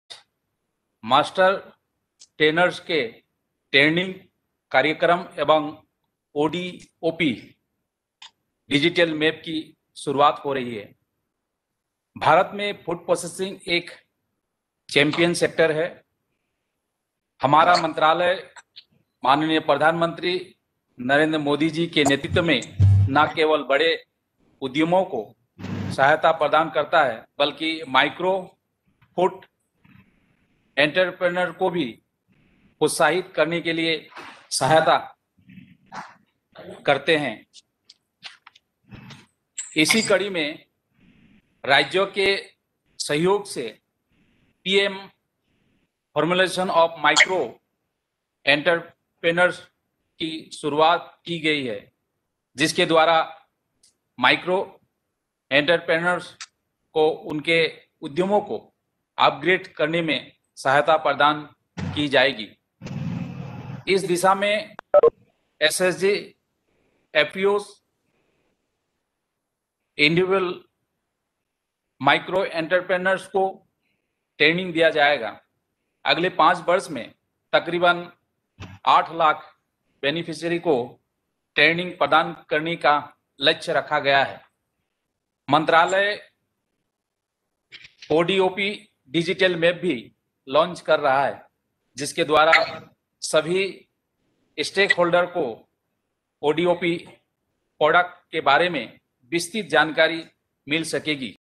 8 लाख उद्यमियों को प्रशिक्षण प्रदान करने का लक्ष्य: केंद्रीय राज्यमंत्री श्री रामेश्वर तेली
राज्यों के सहयोग से पीएम एफएमई योजना के अंतर्गत सूक्ष्म उद्यमियों के उद्यम को अपग्रेड करने में सहायता प्रदान की जाएगी। इसी दिशा में 8 लाख उद्यमियों को प्रशिक्षण प्रदान करने का लक्ष्य रखा गया है:  केंद्रीय राज्यमंत्री श्री रामेश्वर तेली